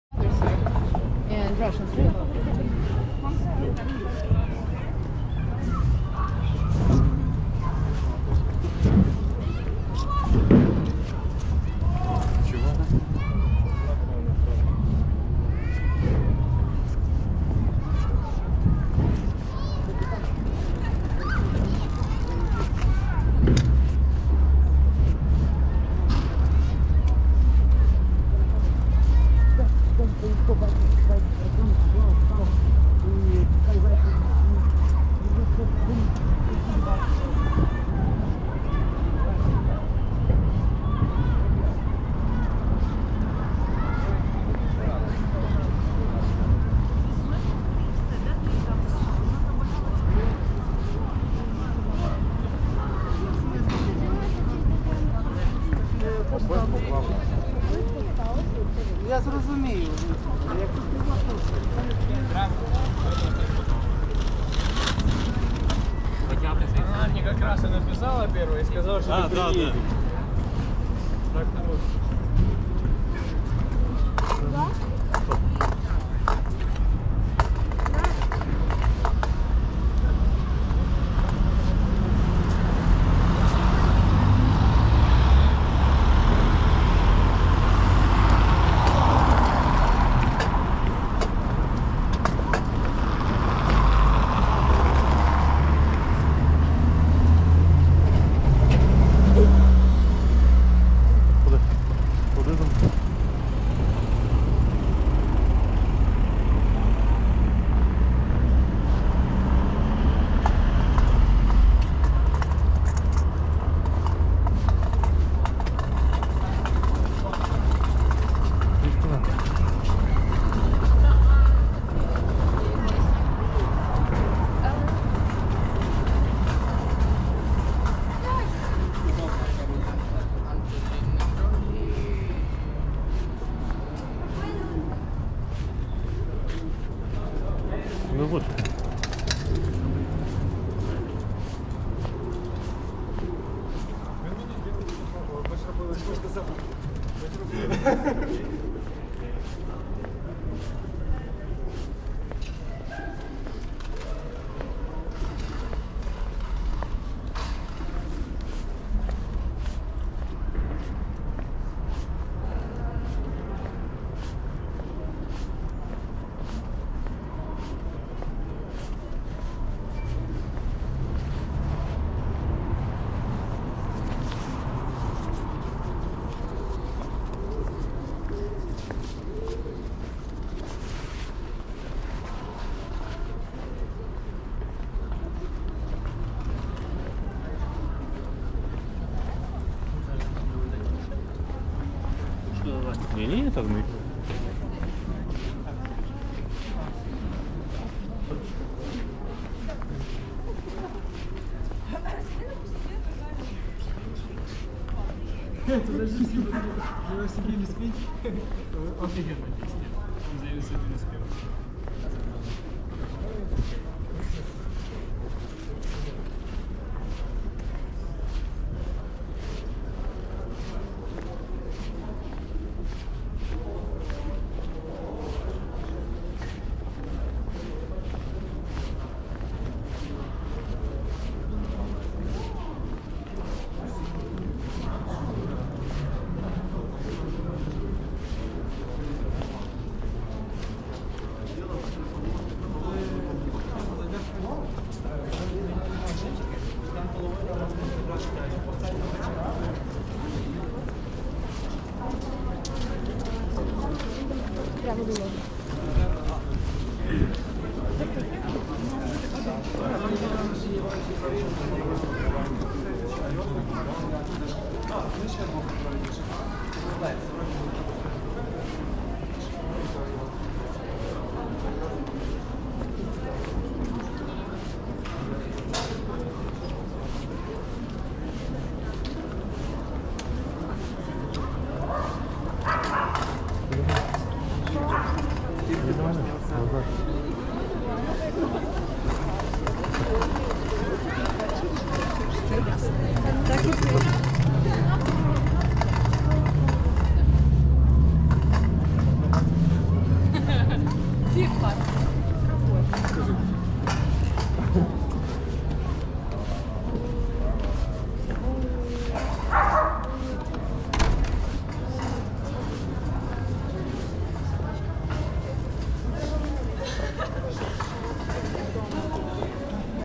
Lviv Ukraine 2023 Travel vlog 4k60fps. Walking tour Львів
00:00 Na Valakh Square
01:59Lesya Ukrainka street
Help the channel to develop. Sign up and put a like.
I am very motivated for new videos. Thank you.
Join us on an exciting journey through the beautiful city of Lviv on our walking tour. This YouTube video will take you on a guided tour of the historic streets of Lviv Old Town and New Town. You'll discover the city's rich history and culture as you stroll through iconic landmarks such as Rynok Square, Bandinelli Palace, Lviv Town Hall

Exploring the beautiful city of Lviv in western Ukraine near the border of Poland.

Exploring LVIV, UKRAINE | This City Is Amazing!